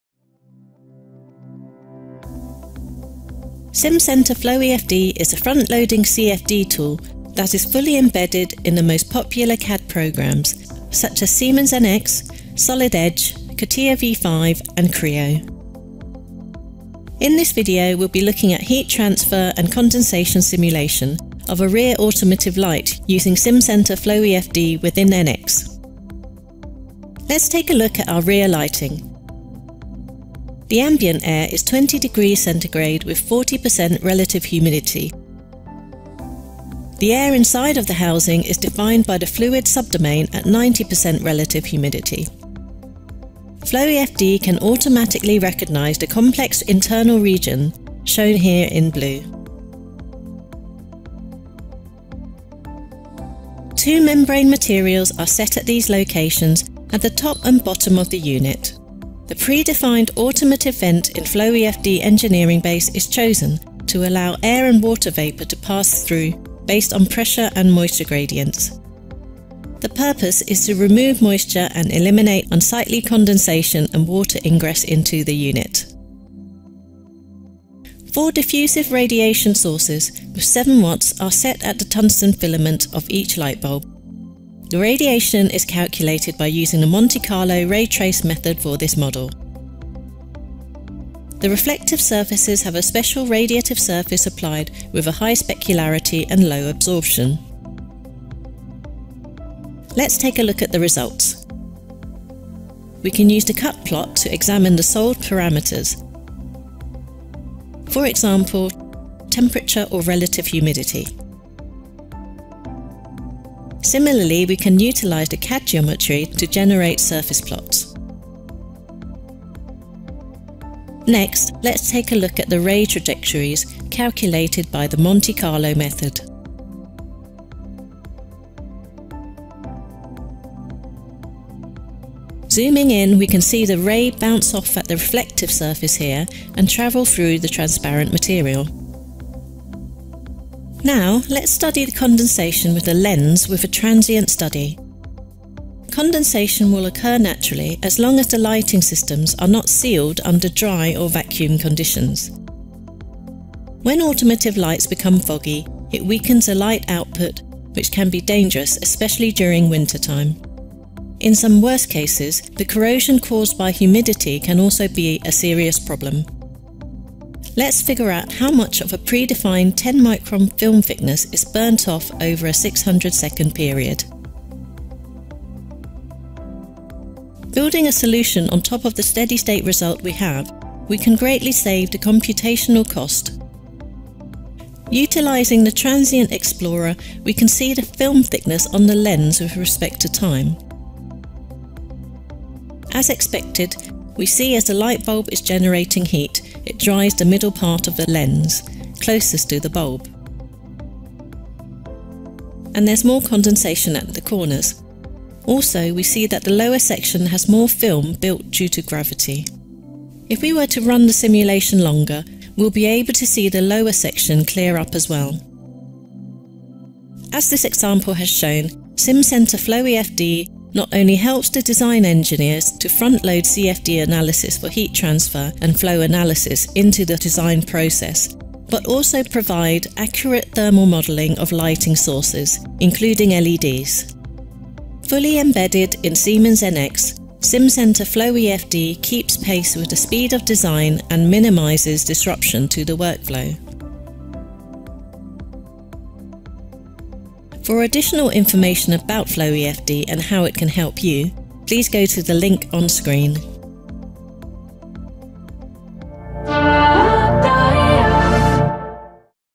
【Simcenter FloEFD for NX】車燈結露-霧氣分析-heat transfer and condensation in automotive lighting兆水代理FloEFD
Simcenter FloEFD 汽車車燈結露分析-Automotive lighting Condensation

兆水科技有限公司
西門子專業工程模擬顧問與軟體代理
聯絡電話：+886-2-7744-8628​